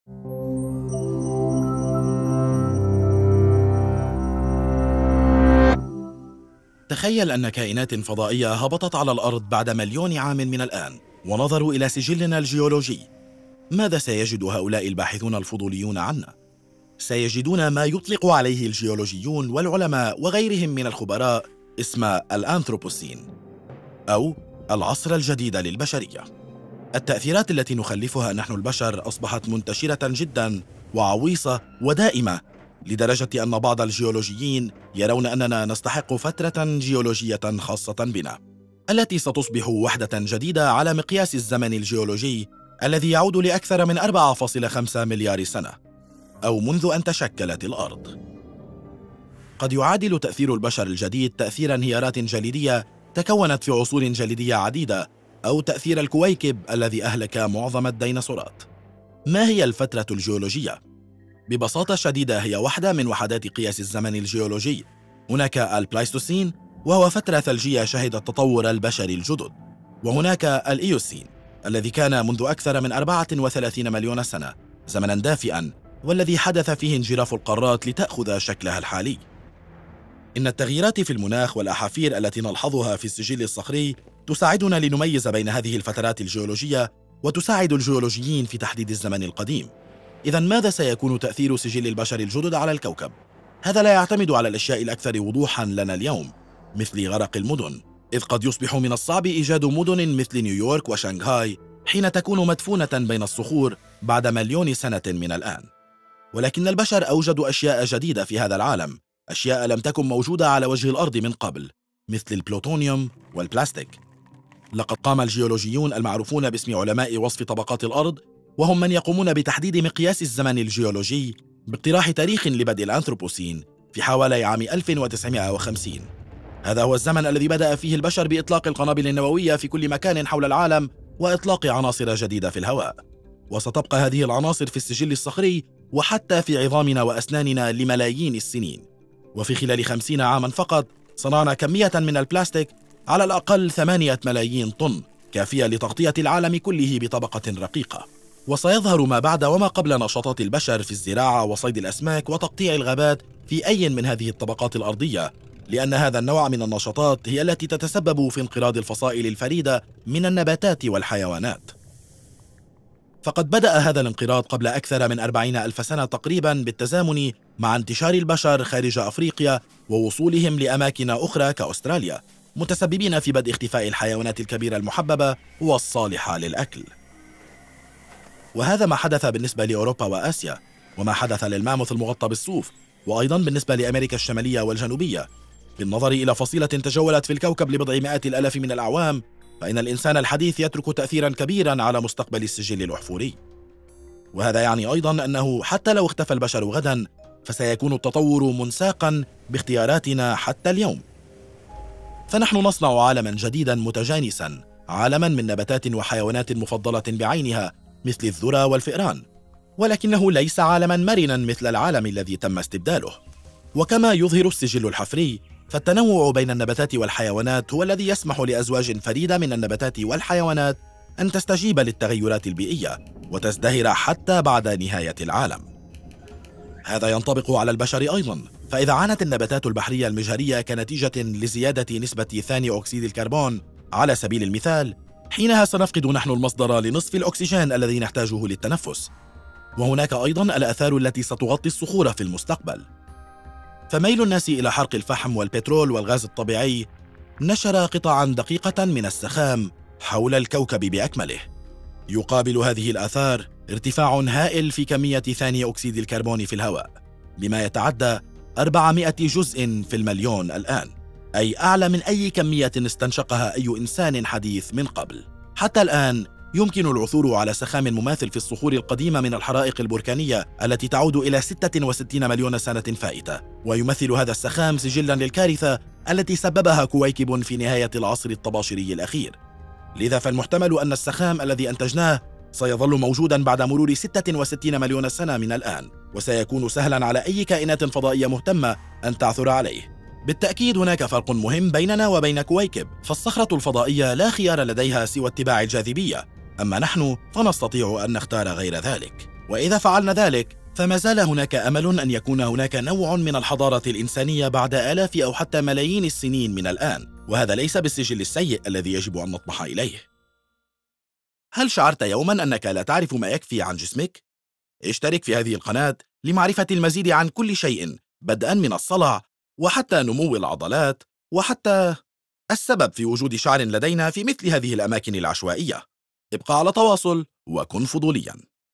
إلى متى ستستمر الآثار البشرية؟ - ديفيد بيلو
تخيّل أن تهبط الكائنات الفضائية على الأرض بعد مليون سنة من الآن. ما الذي سيكتشفه هؤلاء الباحثون الفضوليون عنّا؟ سيجدون ما يُطلق عليه الجيولوجيون، والعلماء، وغيرهم من الخُبراء اسم الأنثروبوسين، أو العصر الجديد للبشرية. يشرح ديفيد بيلو كيف أصبحت الآثار التي أحدثها البشر مُنتشرة، وعميقة، ودائمة لدرجة أن بعض الجيولوجيّين يعتقدون أننا نستحق عصرنا الذي نحياه.

درّسه  David Biello

 إخراج  Margaret To